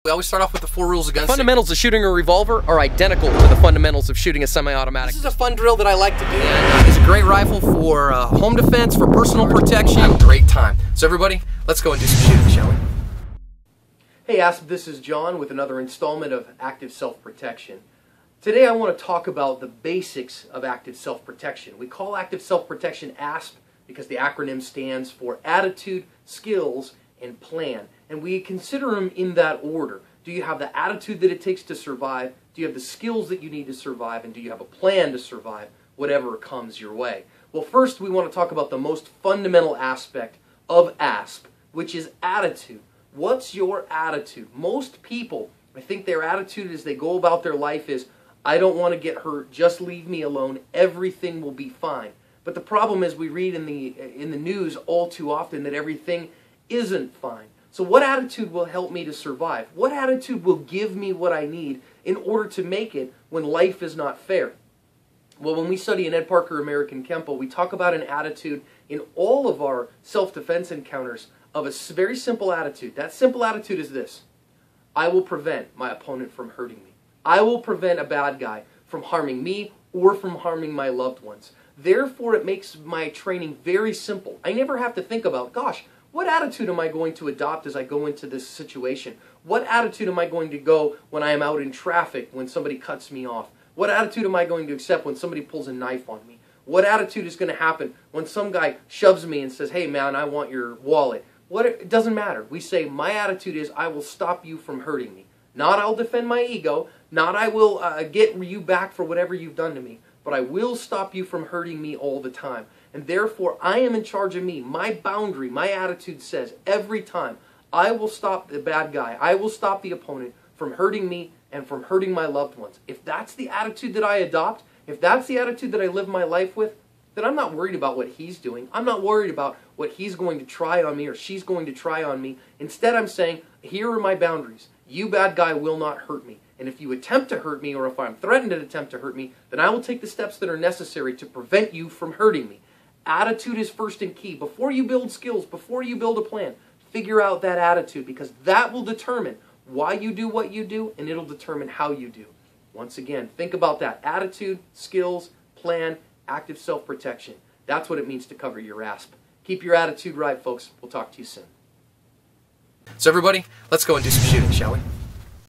Attitude: The First Consideration in Self Defense
John discusses attitude, the fundamental and first consideration in self-defense.